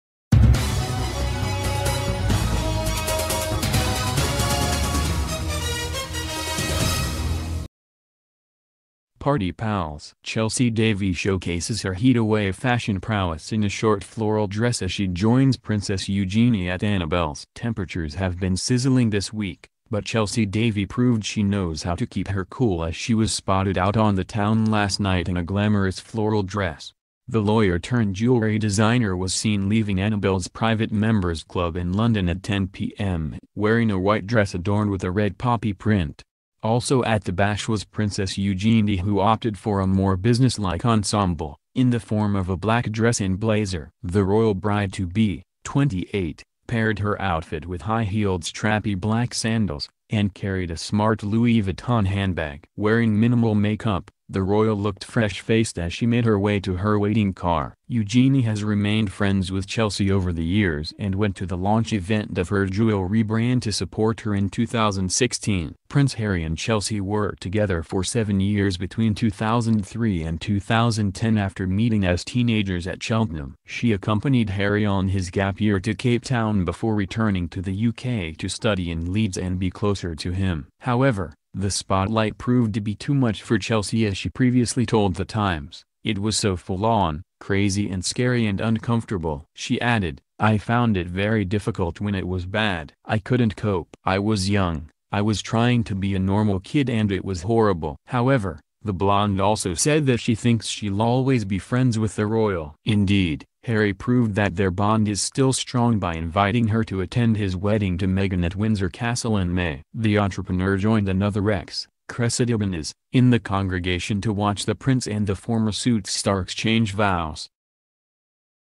Where is Meghan? Chelsy Davy introduces secret romantic dating night with Harry at Annabel's
Party pals! Chelsy Davy showcases her heatwave fashion prowess in a short floral dress as she joins Princess Eugenie at Annabel's